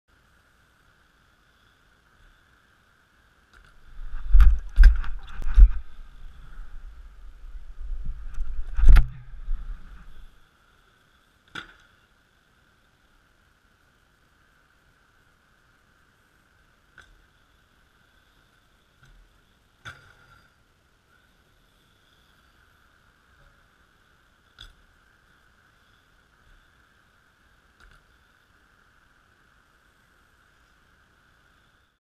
Manual the rail at markeaton
As the title says this is me manualing the rail at markeaton skatepark at dusk :)